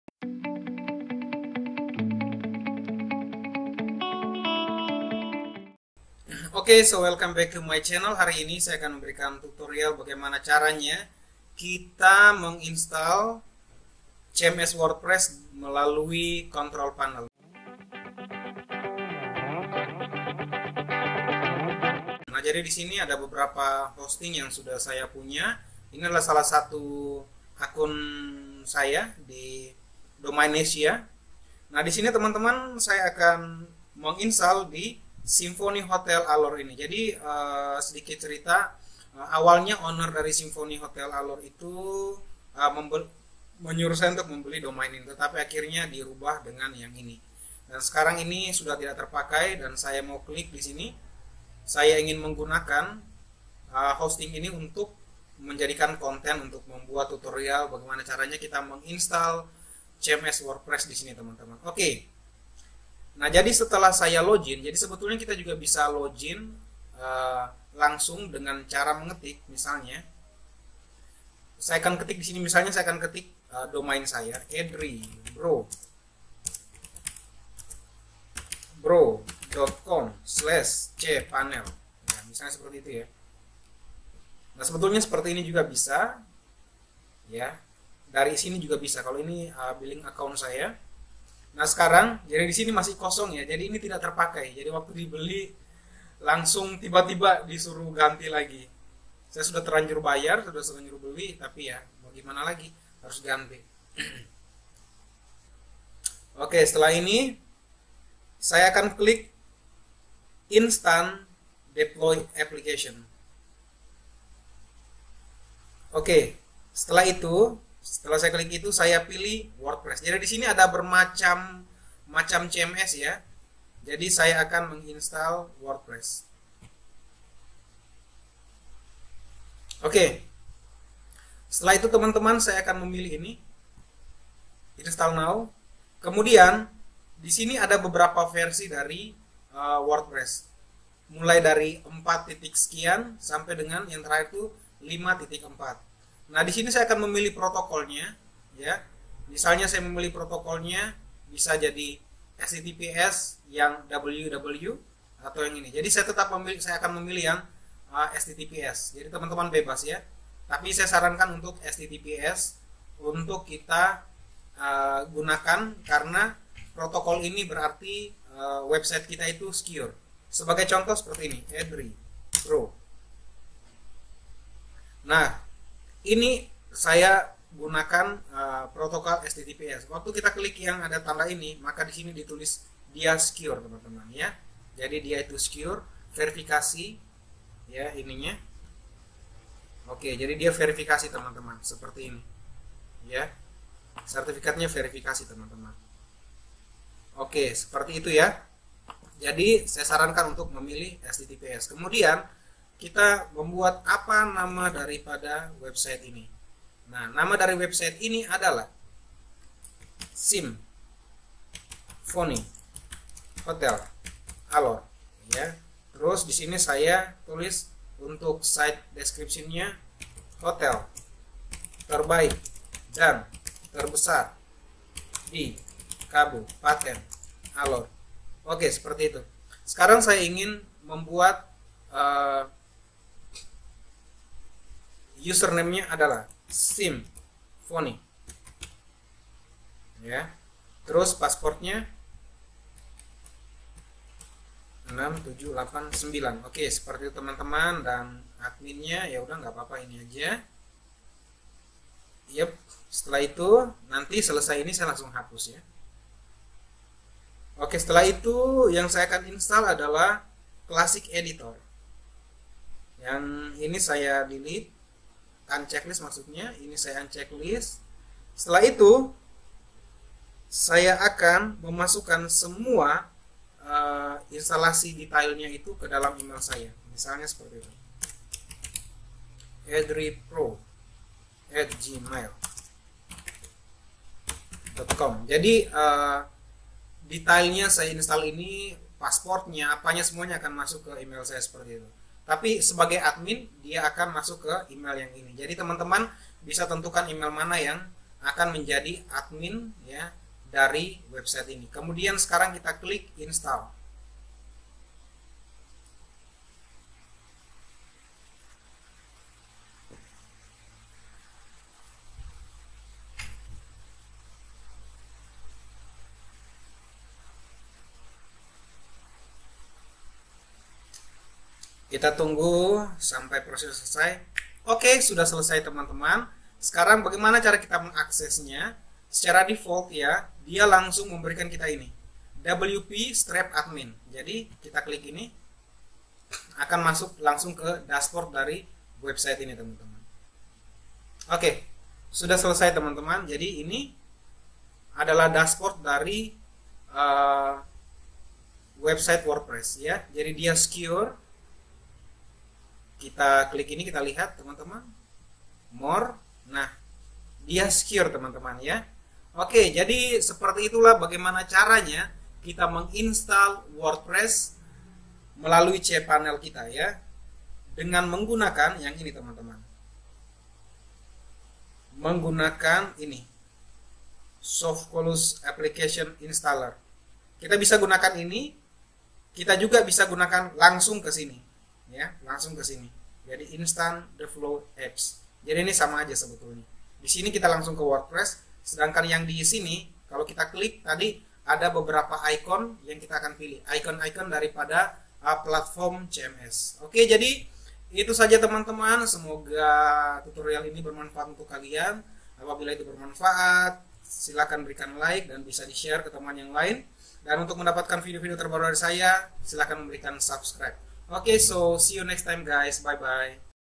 Tutorial Instal WordPress di Cpanel Hosting Domainesia
Video kali ini saya akan menunjukkan bagaimana cara Instal WordPress di Cpanel Hosting Domainesia.